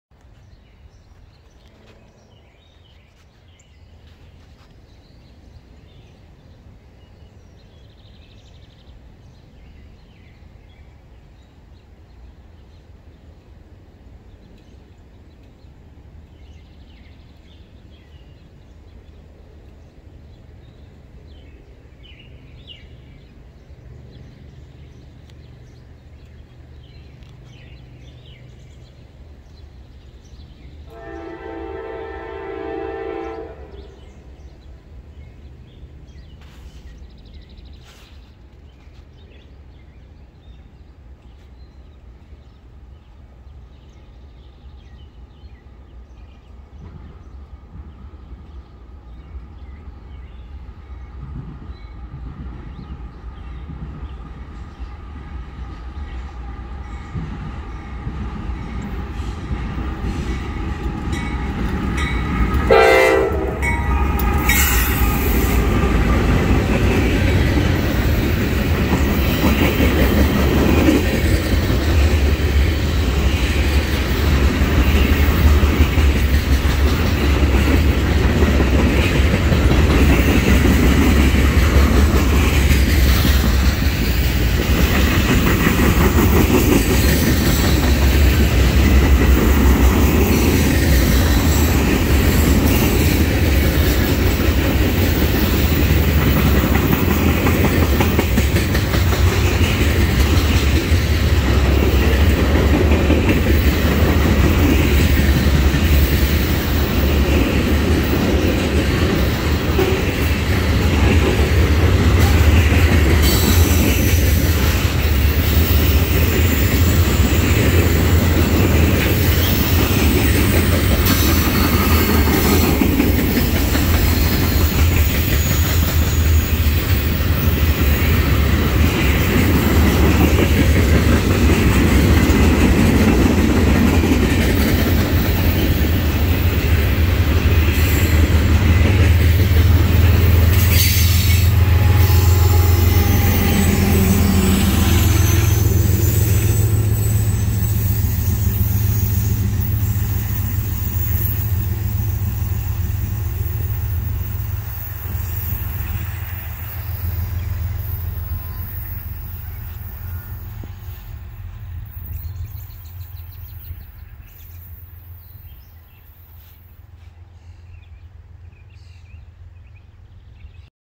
Norfolk Southern #4256 leads a 60 car manifest at Lewistown, PA (06/25/2023)
10G